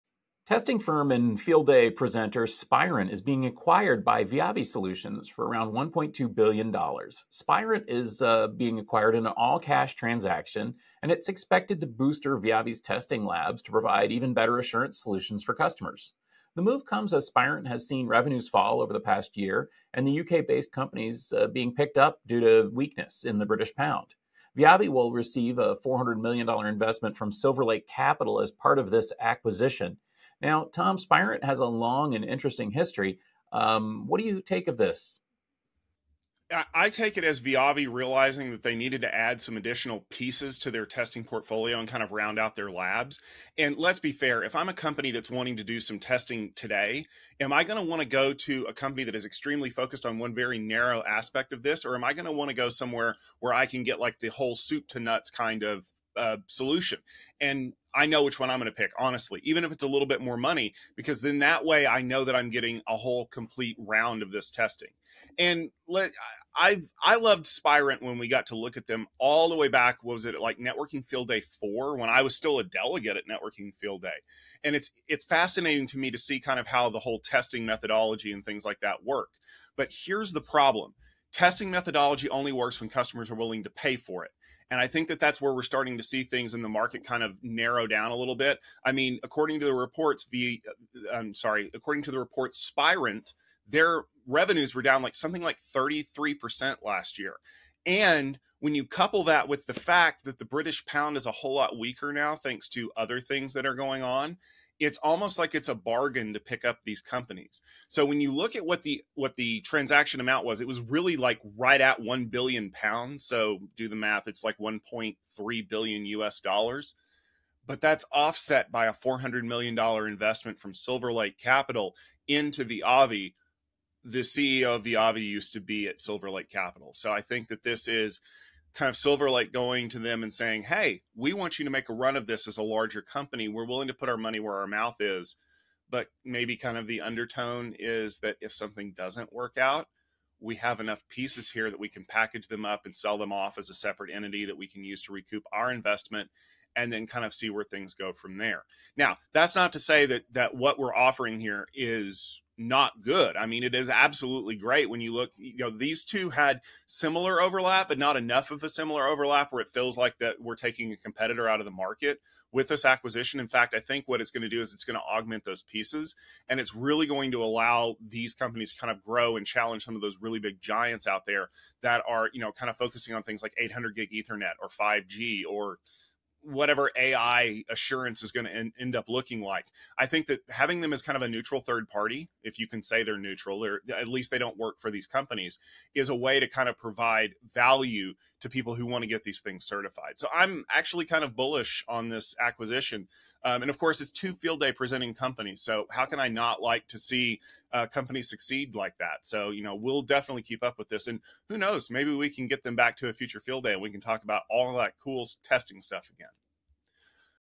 Viavi Acquiring Spirent
Testing firm and Field Day presenter Spirent is being acquired by Viavi Solutions for around $1.2 billion. Spirent is being acquired in an all-cash transaction and is expected to bolster Viavi's testing labs to provide even better assurance solutions for customers. The move comes as Spirent has seen revenues fall over the past year and UK-based companies being picked up due to weakness in the British pound. Viavi will also receive a $400 million investment from Silver Lake Capital as part of the acquisition.